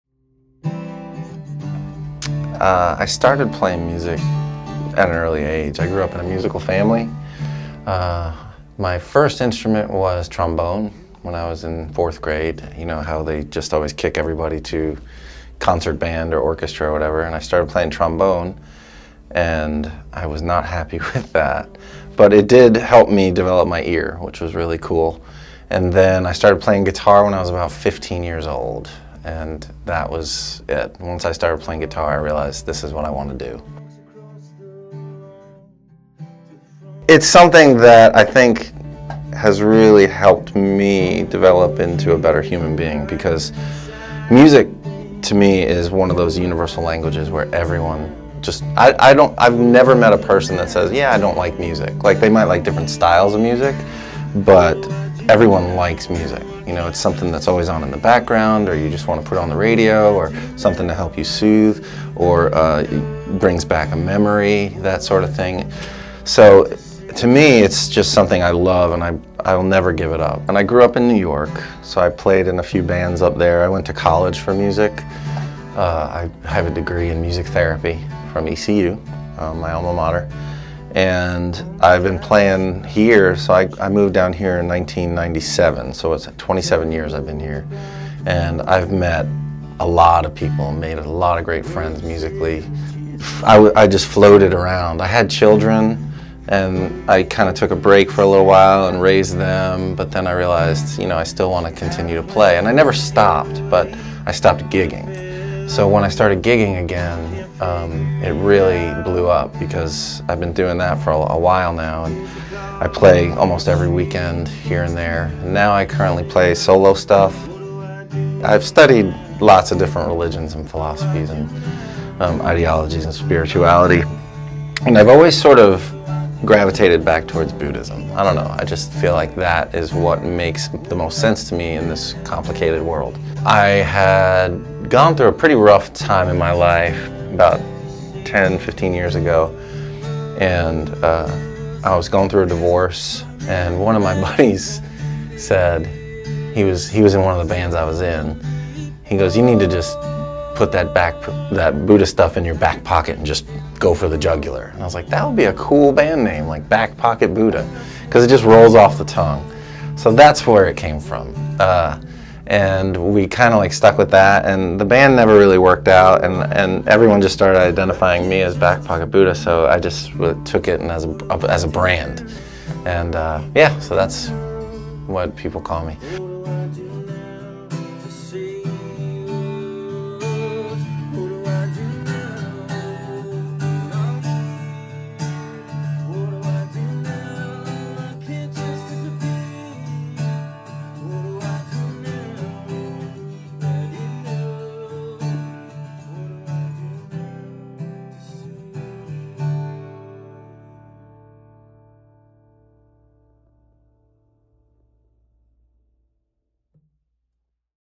WNCT.com’s Music Mondays: Brian Burke
Brian Burke grew up in a musical households and decided guitar was his instrument of choice.